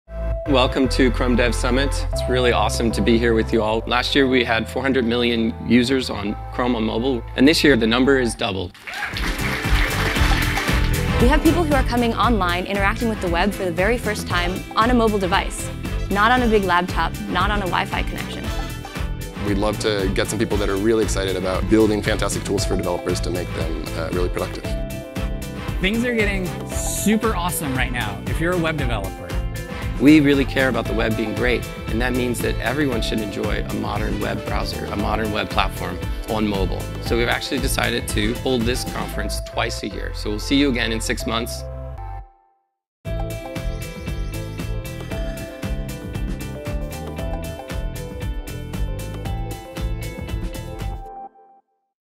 Highlights from Chrome Dev Summit 2015
The Chrome Dev Summit is an exclusive event to connect with Chrome engineers and leading web developers for a two-day exploration of building beautiful and fast mobile web applications.

Learn how to build fast, resilient, app like experiences for the mobile web from Chrome engineers. Take away tips for smooth and fast loading, reliable access on flaky networks and for re-engaging with users.